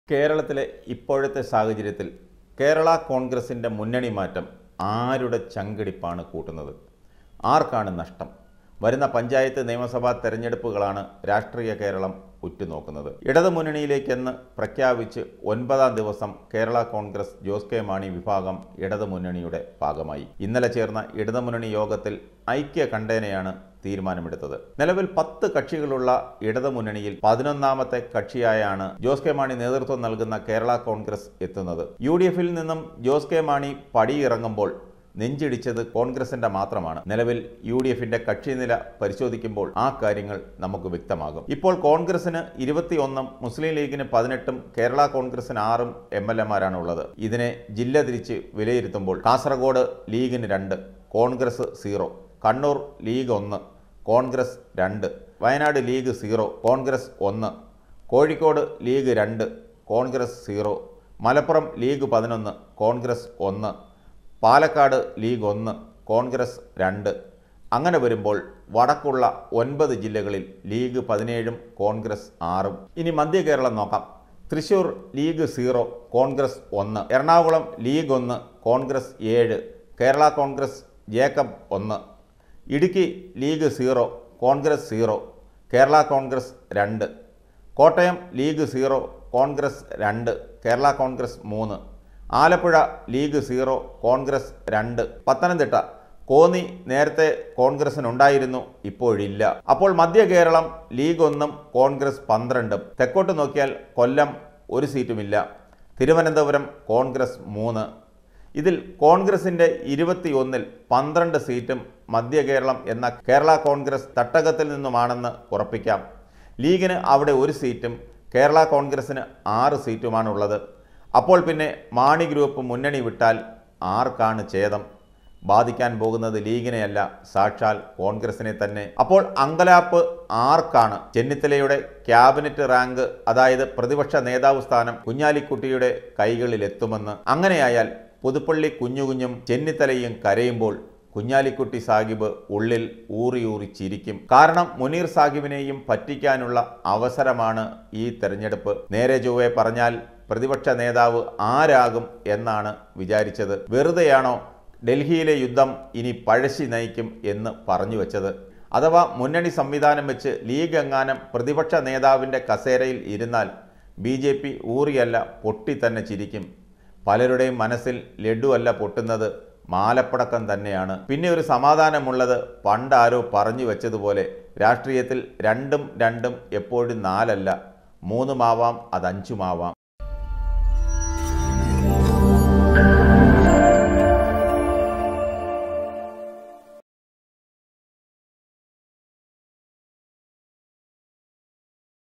ജോസ് കെ മാണിയുടെ മുന്നണി മാറ്റം ആരുടെ നെഞ്ചിടിക്കും ?മനസിൽ ലഡുവല്ല പൊട്ടുന്നത്, മാലപ്പടക്കം തന്നെ
ജോസ് കെ മാണിയുടെ മുന്നണി മാറ്റം ആരുടെ നെഞ്ചിടിക്കും ?
മനസിൽ  ലഡുവല്ല പൊട്ടുന്നത്, മാലപ്പടക്കം തന്നെ